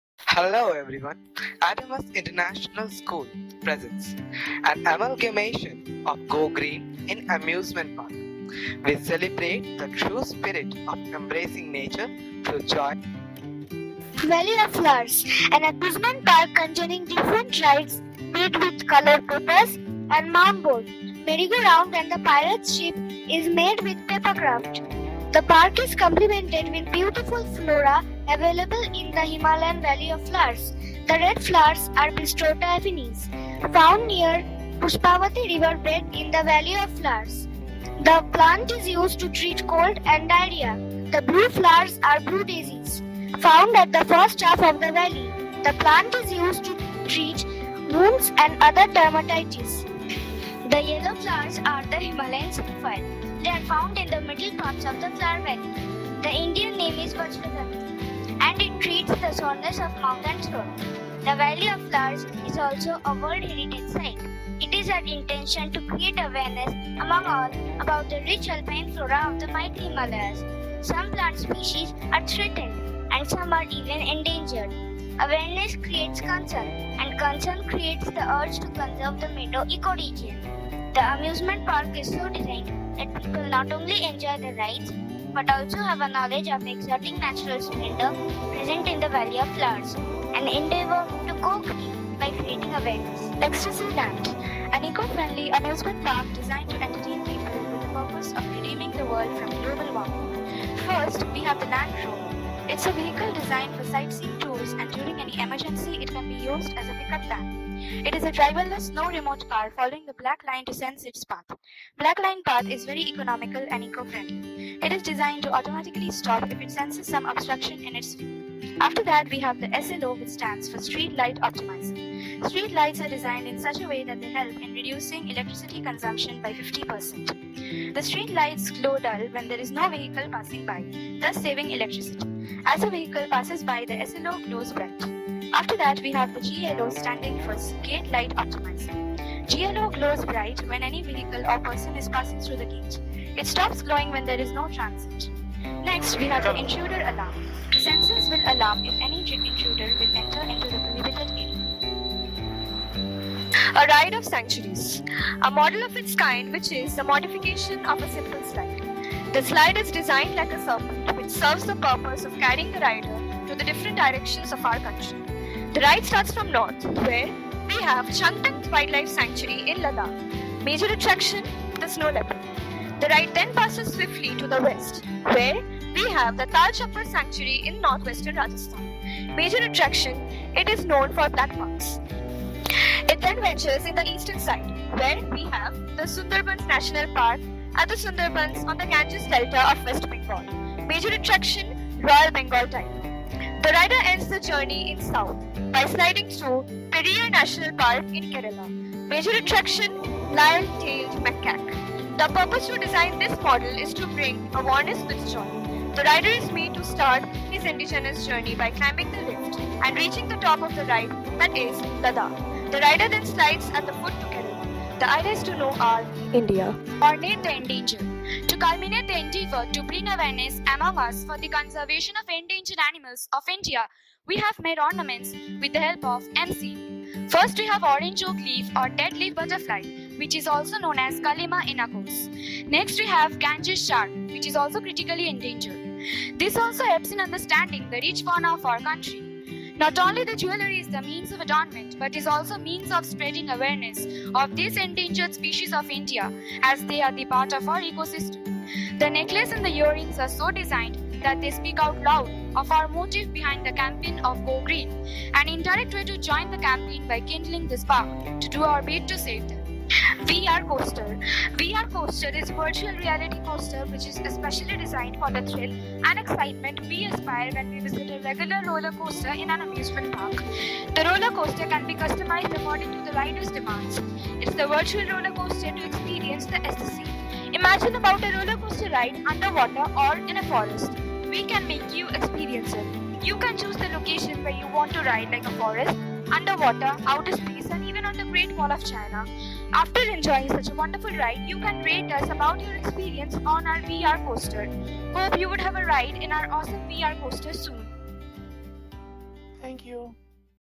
Adamas International School || Student Projects || TINKER Fest 2018
Projects done by students of Adamas International School for Tinker Fest 2018. Students from classes 5 to 11 have participated in the 5 projects under the topics- Paper craft, model making, robotics, digital making and jewellery.  It is our intention to create awareness about the world. Some plant and animal species are threatened and some are even endangered.
Awareness creates concern and concern creates the urge to conserve the meadow eco-region.
The amusement park in Valley of flowers and Ecstasy Land are so designed, that people not only enjoy the rides but also have a knowledge of exotic natural splendor present in Valley of flowers. Ecstasy Land is an eco friendly amusement park designed to entertain people with a purpose of redeeming the World from Global warming.
A ride to sanctuaries gives information about the Sanctuaries of India. A model of its kind which is the modification of a simple slide. The slide is designed like a serpent which serves the purpose of carrying the rider to the different directions of our country.
Ornate the endangered is to culminate our endeavour to create awareness for the conservation of endangered animals of India, by creating ornaments with the help of M-seal.
VR coaster is a GIF (Graphics Interchange Format) of a roller coaster ride where the rider can customise the ride according to his/ her wish and enjoy the thrill and excitement we aspire when we visit a regular Roller Coaster in an amusement park.